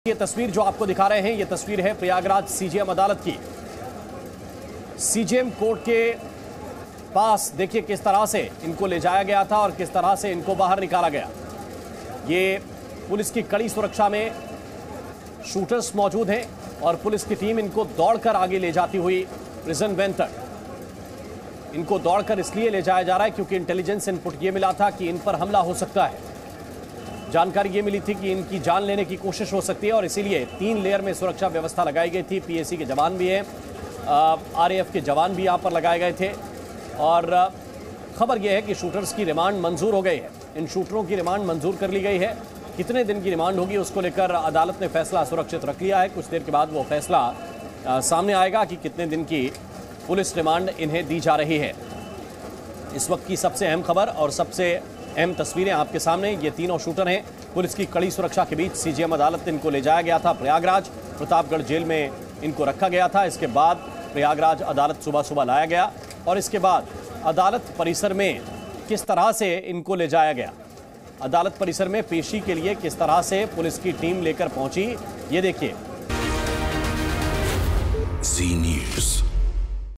Atiq-Ashraf Murder: माफिया ब्रदर्स के Shooters को लाया गया Prayagraj CJM Court, Police Remand मंज़ूर
अतीक और अशरफ हत्याकांड मामले में आज तीनों आरोपी शूटरों को प्रयागराज की CJM कोर्ट में लाया गया। बता दें कि अदालत परिसर में शूटरों को भगा-भागकर ले जाया गया। कोर्ट में पेशी के दौरान पुलिस रिमांड की मंजूरी दे दी गई है।

 In the Atiq and Ashraf murder case, today all the three accused shooters were brought to the CJM court of Prayagraj. Shooters were taken away in the court premises. Police remand has been approved during the production in the court.